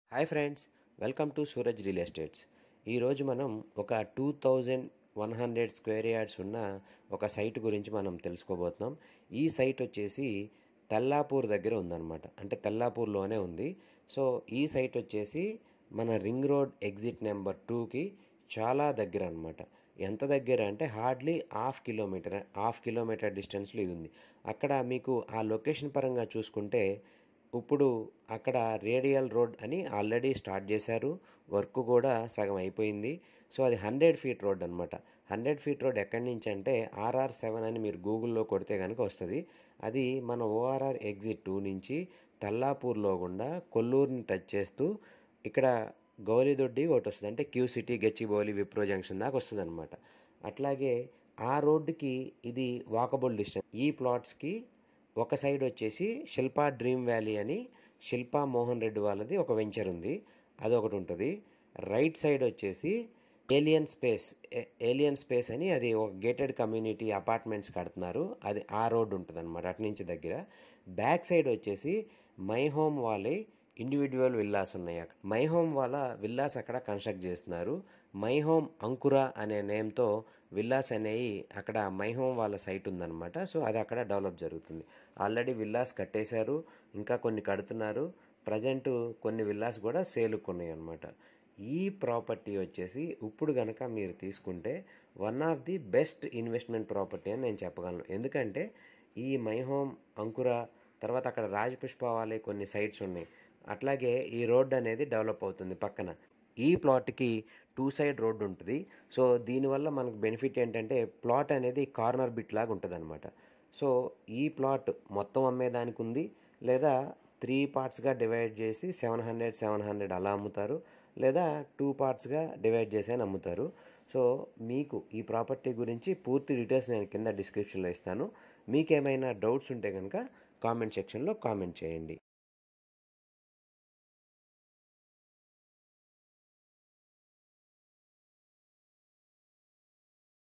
Residential land / Plots in Tellapur Hyderabad for Sale l Open Plots for sale In Tellapur Hyderabad
Residential land and Plots in Tellapur Hyderabad for Sale

Property Details : -

Total sq Yards: 2100 Sq yards
Location : Tellapur
Land Mark : Near ORR (R7 100 Feet Road ) beside Silpa dream valley
Saying Price : 38,000/- per sq yard
Final Price : 36,000/- per sq yard owner is in some need of money.

Plot details :-

Facing : Yes
2 Side Road Facing
East :114
West : 98  ( Road FACE Road Size 13 Feet )
North : 184
South : 186 ( Road FACE Road Size 30 Feet )
LRS : Not Available as of now, Owner will take care about this before registration
ULC  ( Urban Land Ceiling) : Cleared

Location details : -

Locality Name : Tellapur ( తెల్లాపూర్ )
City Name : Ramchandrapuram
District : Medak
State : Telangana
Region : Telangana

0.5 km from ORR exit 2
0.5 km from RR7 exit 2
Beside Silpa Dream Valley
Beside my Home Ankura Villas

Suraj Real Estates
Madhapur Hyderabad, Hyderabad, Telangana 500081